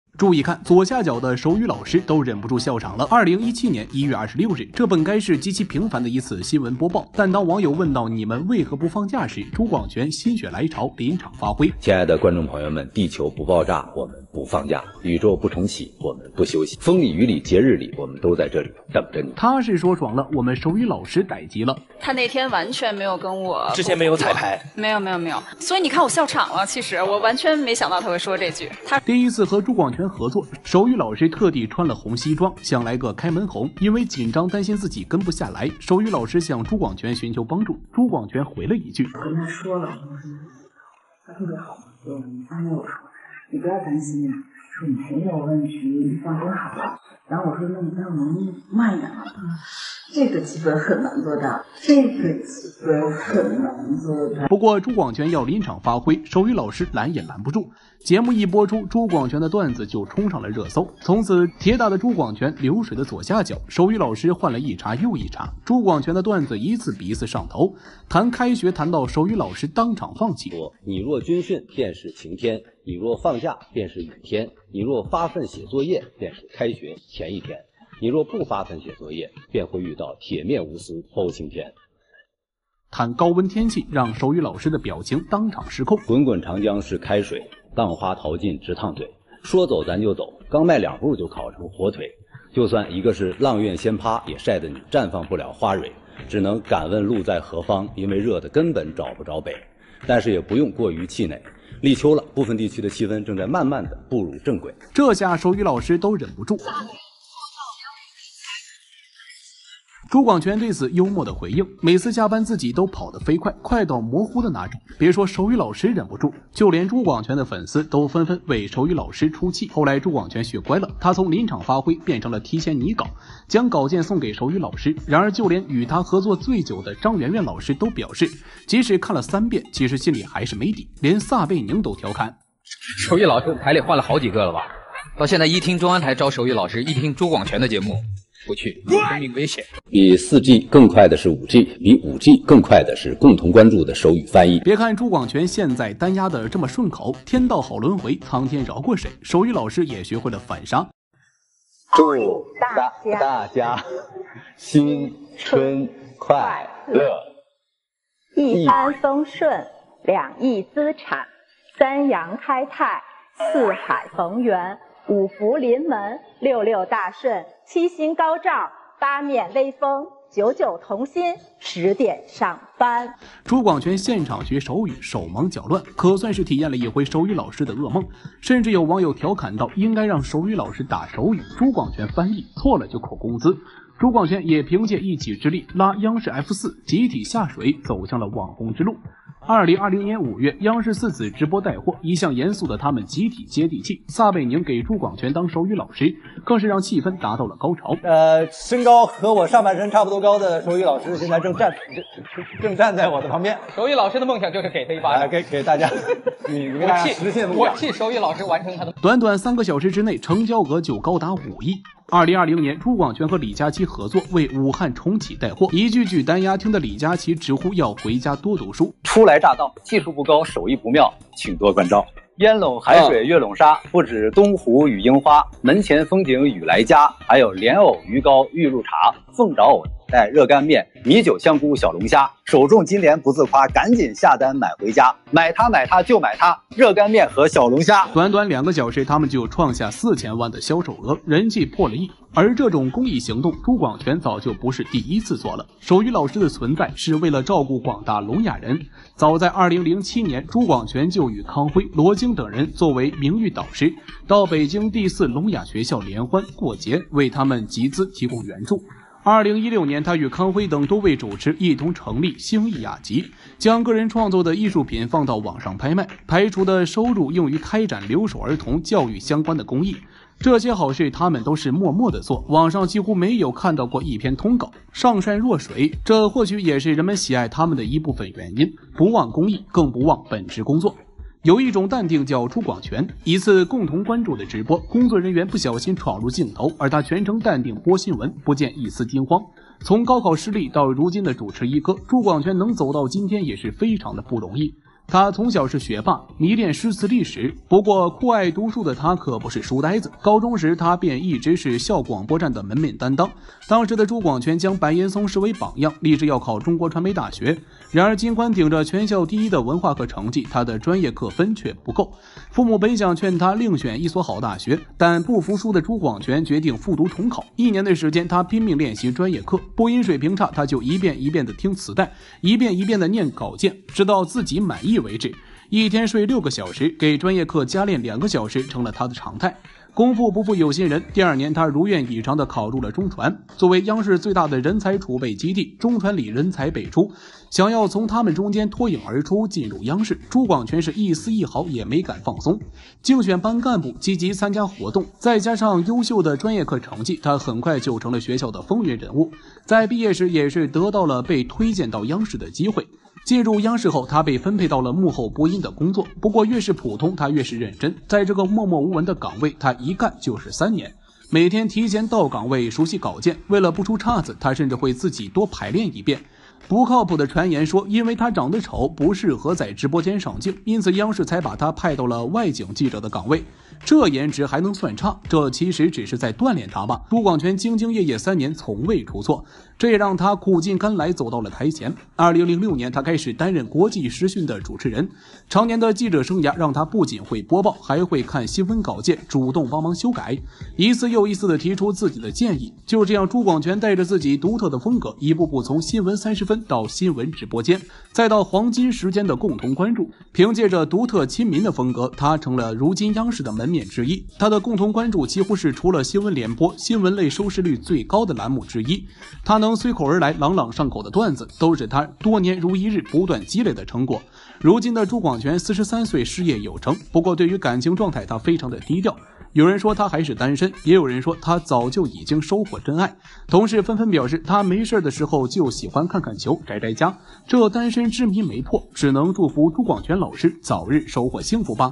押韵狂魔朱广权高考失利，进入央视后坐了三年冷板但，一个段子全网爆火【叮当不吃娱】
大家好，这里是“叮当不吃娱”
本频道致力于创造一个友好、和平的环境，绝对不会容忍任何形式的仇恨言论。
带你了解不同网红人的故事